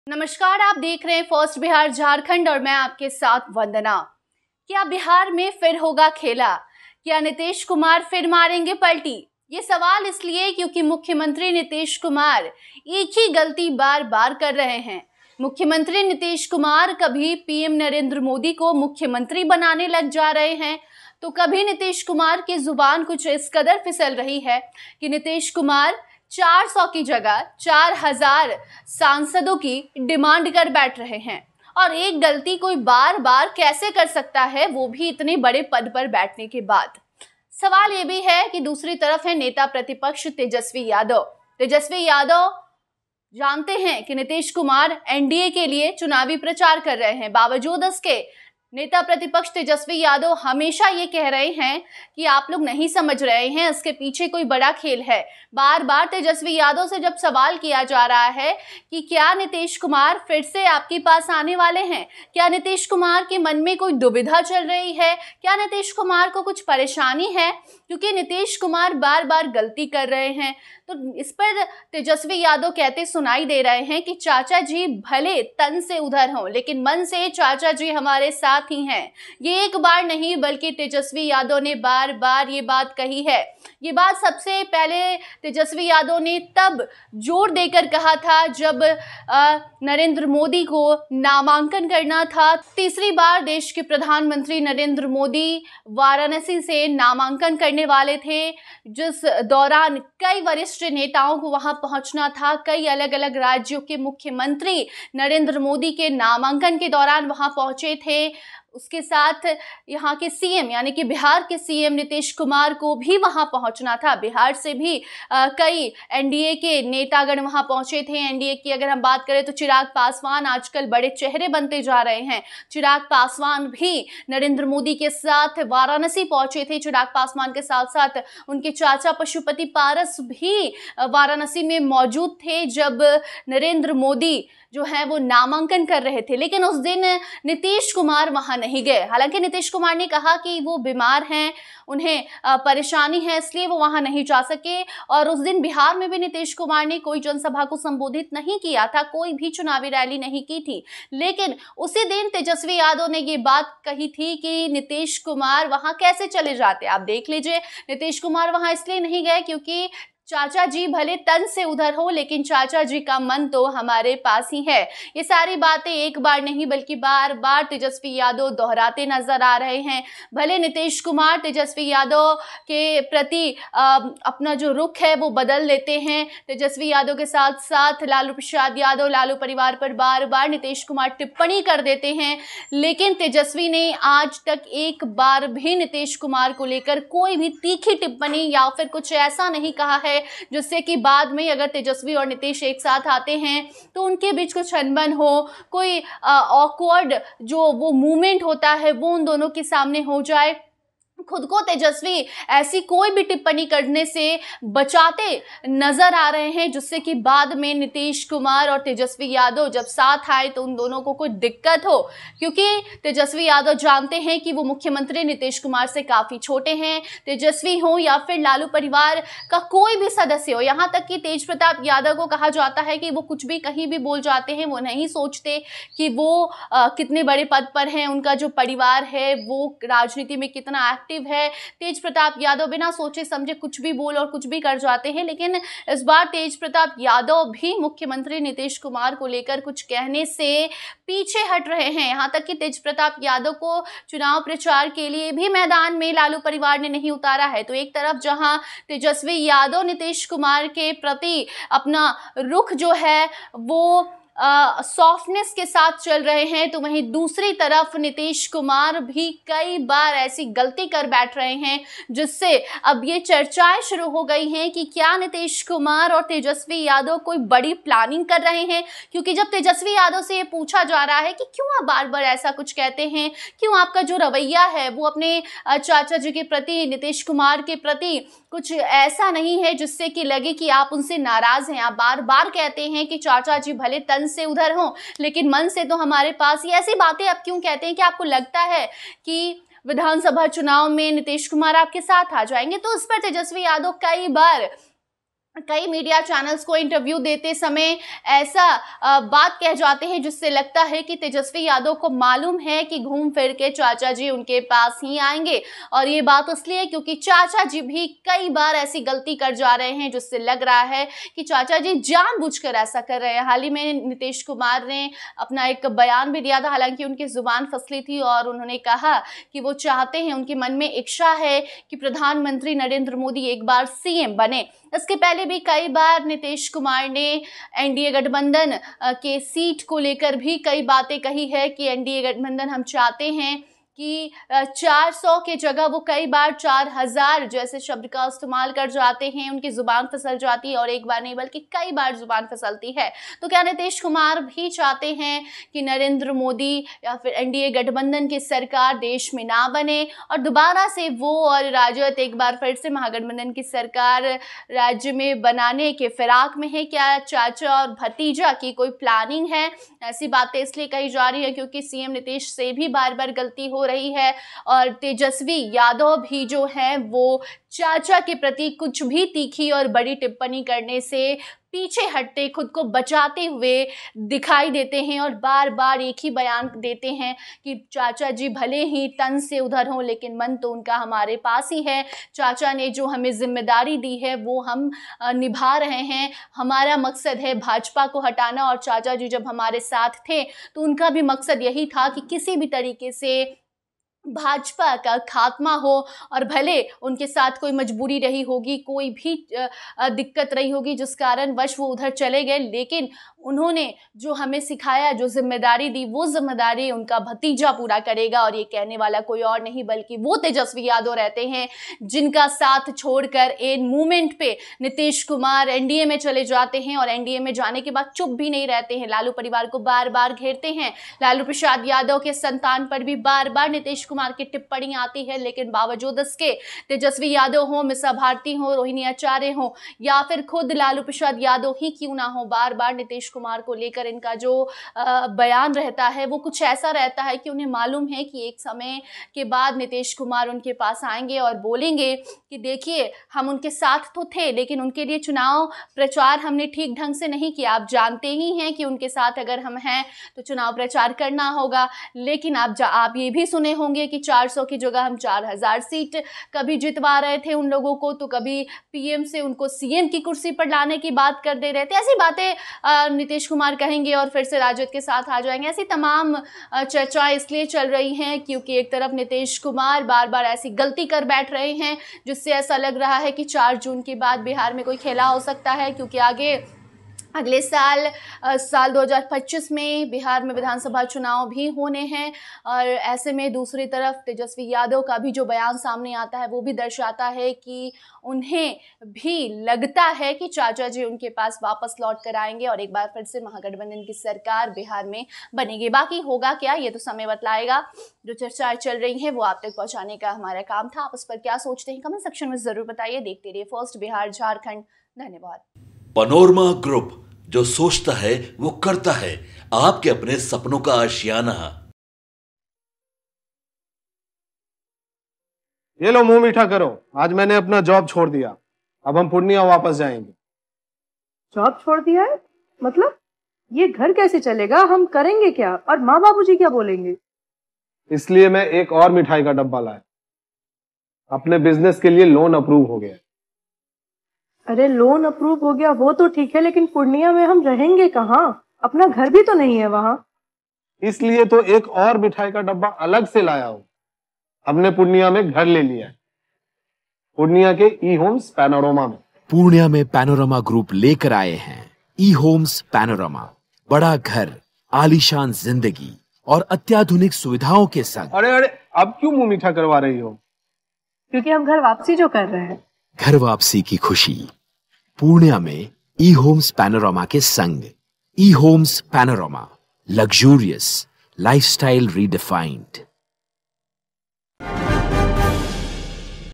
Nitish -Tejashwi के जाल में फंस गए PM Modi, Bihar में 4 June के बाद फिर हो सकता है खेल!
नीतीश कुमार और तेजस्वी यादव के जाल में फंस गए पीएम मोदी, बार - बार नीतीश की गलती कोई बड़ा इशारा कर रही,क्या 4 जून के बाद होगा खेला?

Other Trending Videos:

1.
2.
3.